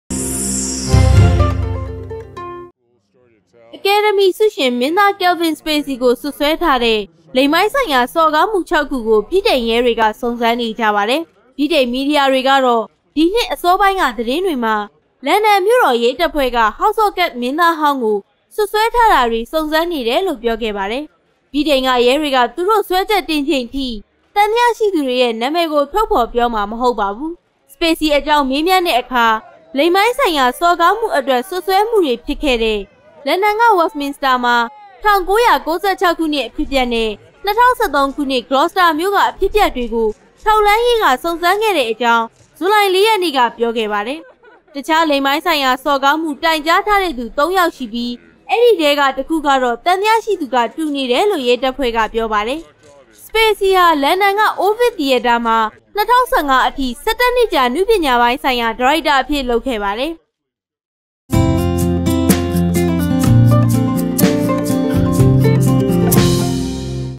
ကယ္ဗင္ စေပစီကိုၿဗိတိန္ရဲေတြ စံုစမ္းစစ္ေဆးေနၿပီ
“အကယ္ဒမီဆုရွင္ မင္းသား ကယ္ဗင္ စေပစီကို စြပ္စြဲထားတဲ့  လိင္ပိုင္းဆုိင္ရာေစာ္ကားမႈ ေျခာက္ခုကို  ၿဗိတိန္ရဲေတြက စံုစမ္းေနၾကပါတယ္။”